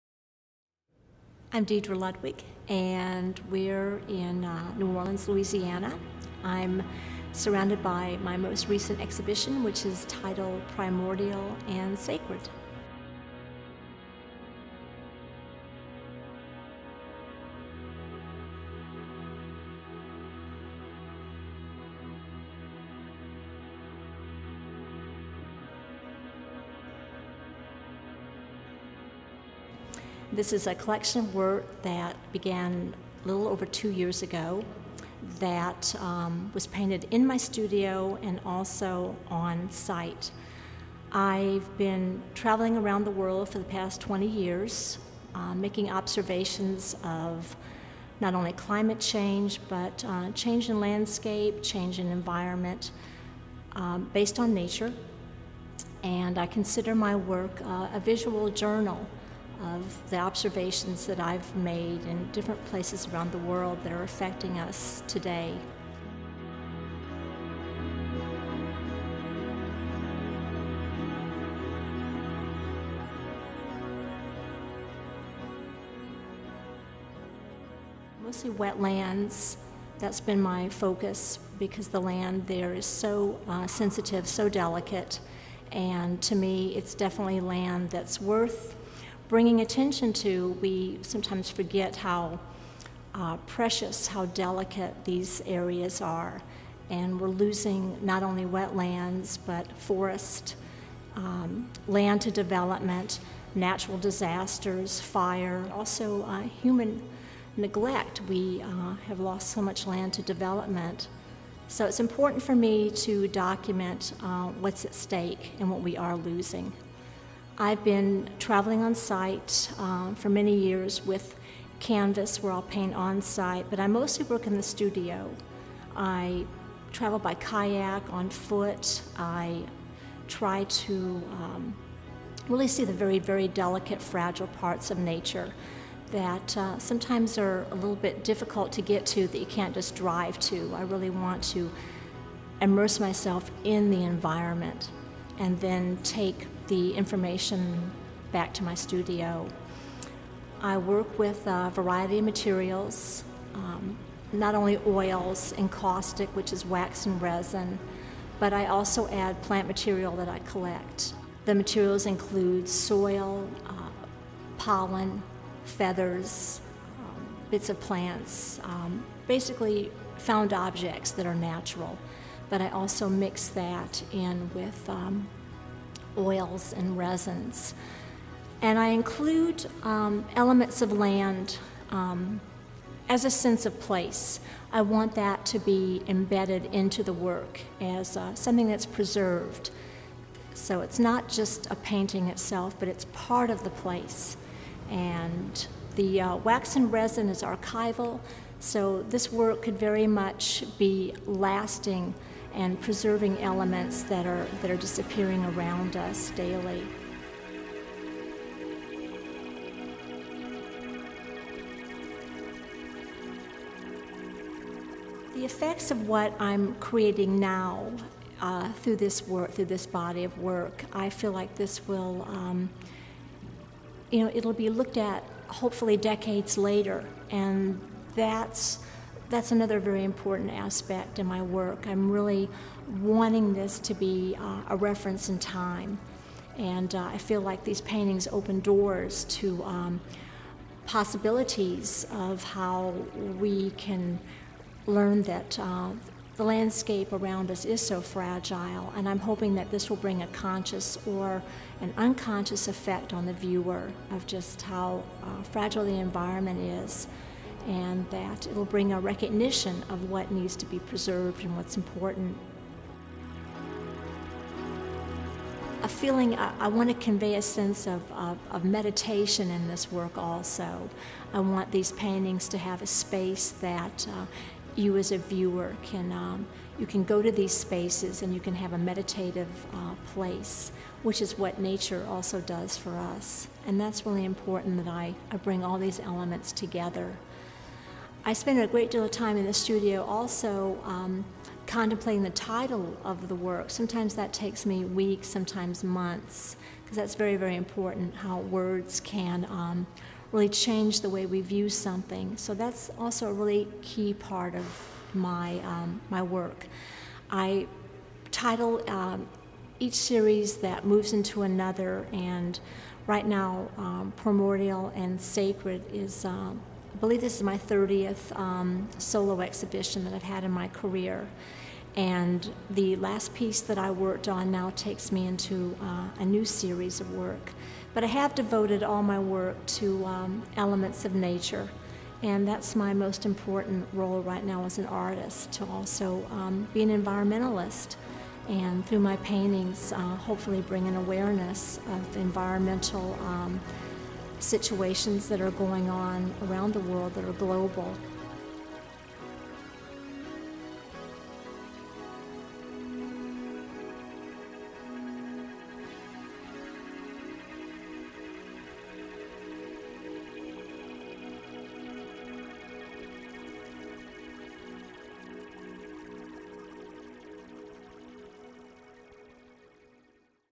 Deedra Ludwig "Primordial and Sacred" at LeMieux Galleries
Deedra Ludwig talks about her show "Primordial and Sacred" at LeMieux Galleries in New Orleans, March 2012.